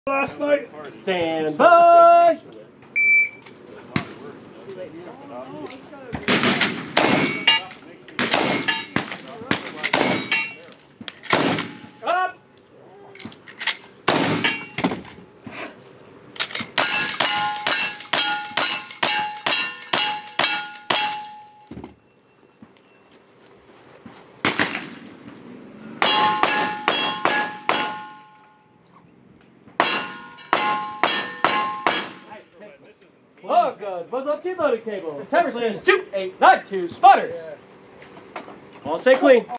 Stage 2 Polecat Outlaws 9 25 21
OK Corral Outlaws Septembert Monthly Match at the OK Corral Gun Club in Okeechobee, FL.
Special Thanks to JP Law, Midwest Hale for helping film.

Top 5 Overall:
1) Arcadia Outlaw - 49er - 115.77
2) The Outlaw Travis James - WRA - 139.63
3) Crane Creek Kid - COWB - 140.00
4) JP Law - WRA - 140.14
5) Jeremiah Longknife - ES - 143.64

Top Ladies:
1) Midwest Hale COWG
2) Velvet Sheath LS
3) Chickie Winsome LWRA

Clean Shooters:
Jeremiah Longknife

Dagnabbit:
Arcadia Outlaw